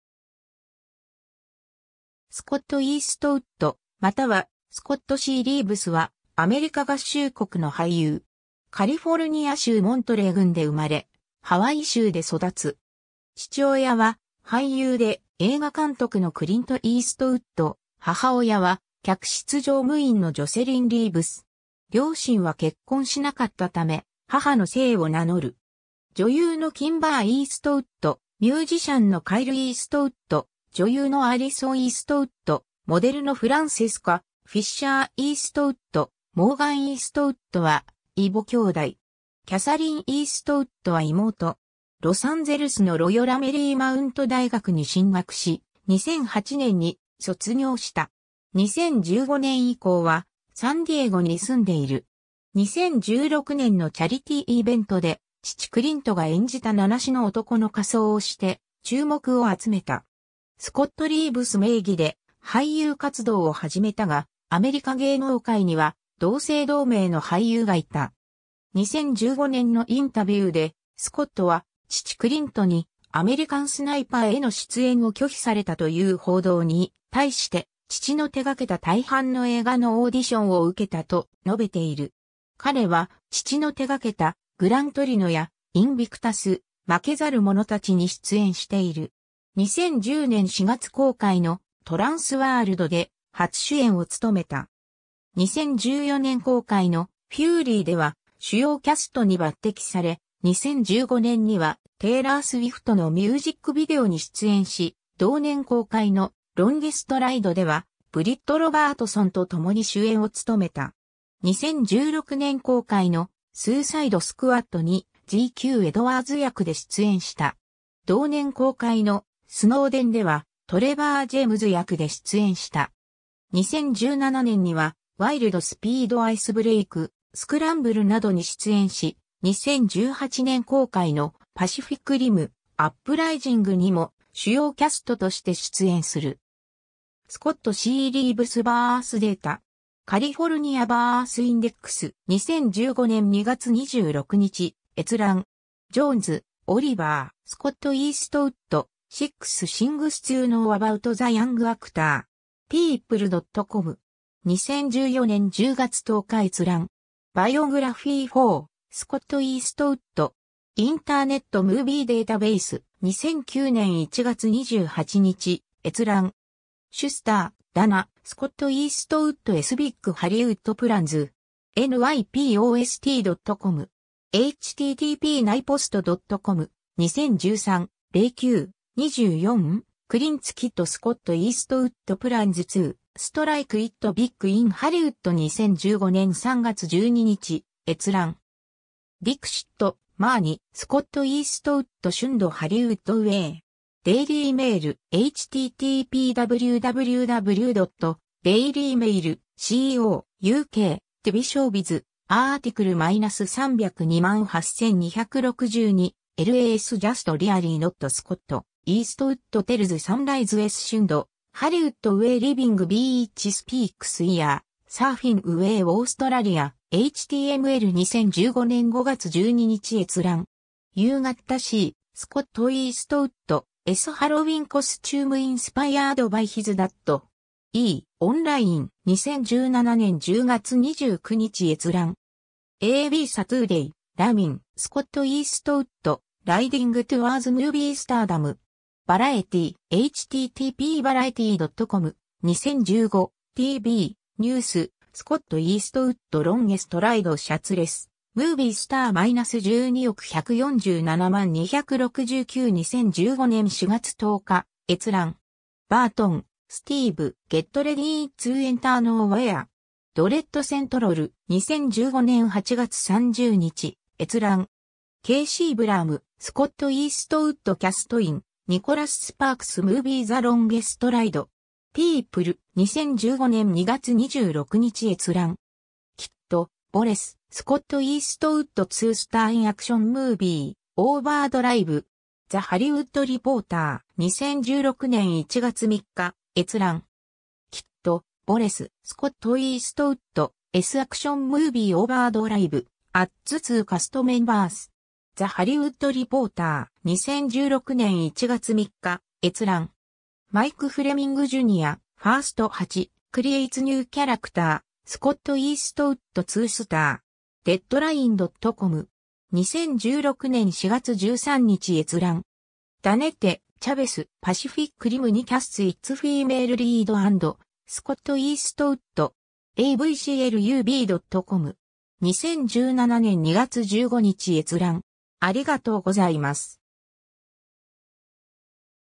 スコット・イーストウッド
クリント・イーストウッド
スコット・イーストウッド（Scott Eastwood, 1986年3月21日 - ）またはスコット・C・リーヴス（Scott Clinton Reeves）は、アメリカ合衆国の俳優。
 カリフォルニア州モントレー郡で生まれ、ハワイ州で育つ。
父親は俳優で映画監督のクリント・イーストウッド、母親は客室乗務員のジョセリン・リーヴス。
両親は結婚しなかったため、母の姓を名乗る。
女優のキンバー・イーストウッド、ミュージシャンのカイル・イーストウッド、女優のアリソン・イーストウッド、モデルのフランセスカ・フィッシャー・イーストウッド、モーガン・イーストウッドは異母兄弟。
キャサリン・イーストウッドは妹。
 ロサンゼルスのロヨラ・メリーマウント大学に進学し、2008年に卒業した。
2015年以降はサンディエゴに住んでいる。
2016年のチャリティーイベントで父クリントが演じた名無しの男の仮装をして注目を集めた。
 「スコット・リーヴス」名義で俳優活動を始めたが、アメリカ芸能界には同姓同名の俳優がいた。
2015年のインタビューで、スコットは「父クリントに『アメリカン・スナイパー』への出演を拒否された」という報道に対して、「父の手掛けた大半の映画のオーディションを受けた」と述べている。
彼は父の手掛けた『グラン・トリノ』や『インビクタス/負けざる者たち』に出演している。
 2010年4月公開の『トランス・ワールド』で初主演を務めた。
2014年公開の『フューリー』では主要キャストに抜擢され、2015年にはテイラー・スウィフトのミュージックビデオに出演し、同年公開の『ロンゲスト・ライド』ではブリット・ロバートソンと共に主演を務めた。
2016年公開の『スーサイド・スクワッド』にGQ・エドワーズ役で出演した。
同年公開の『スノーデン』ではトレバー・ジェイムズ役で出演した。
2017年には『ワイルド・スピード ICE BREAK』『スクランブル』などに出演し、2018年公開の『パシフィック・リム: アップライジング』にも主要キャストとして出演する。
 ^ “Scott C. Reeves birth data”. California Birth Index. 2015年2月26日閲覧。
 ^ Jones, Oliver (2013年9月30日). “Scott Eastwood: Six Things to Know About the Young Actor”. People.com. 2014年10月10日閲覧。
 ^ “Biography for Scott Eastwood”. Internet Movie Database. 2009年1月28日閲覧。
^ Schuster, Dana (September 24, 2013). “Scott Eastwood's Big Hollywood Plans”. Nypost.com. 2015年3月12日閲覧。
^ Dixit, Marni (2015年4月7日). “Scott Eastwood shunned Hollywood way”. Daily Mail (London). 2015年5月12日閲覧。
 ^ “You Gotta See Scott Eastwood's Halloween Costume Inspired by His Dad”. E! Online. 2017年10月29日閲覧。
^ a b Setoodeh, Ramin (2015年4月10日). “Scott Eastwood, Riding Towards Movie Stardom”. Variety.